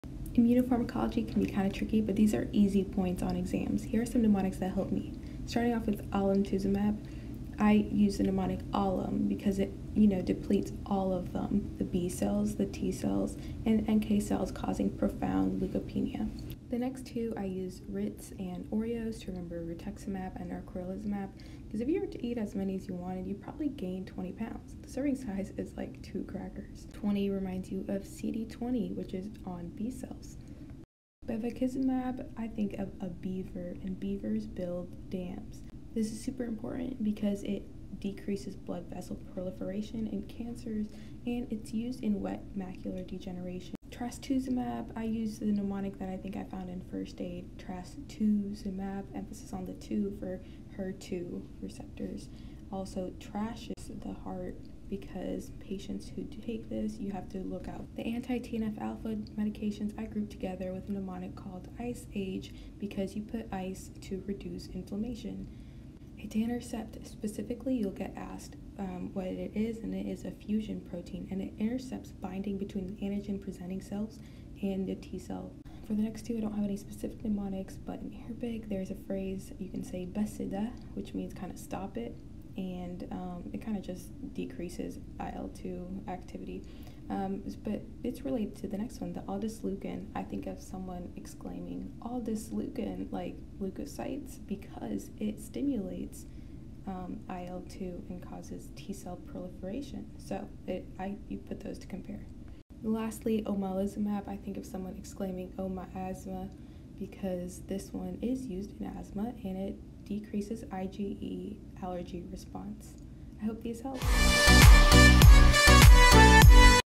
Quick mnemonics for immunopharmacology-alemtuzumab, bevacizumab, omalizumab etc. |USMLE
Easy mnemonics for immunopharmacology!